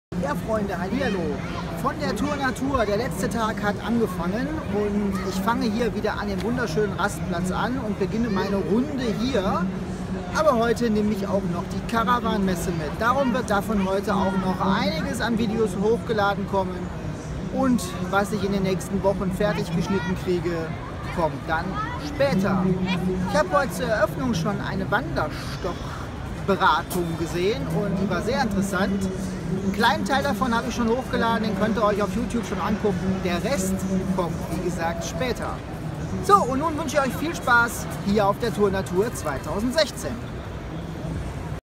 Tour Natur 2016 (Der letzte Messetag hat angefangen)
Heute der letzte Messetag der Tour Natur 2016 ich versuche viel zu Filme und mit vielen Leuten zu Sprechen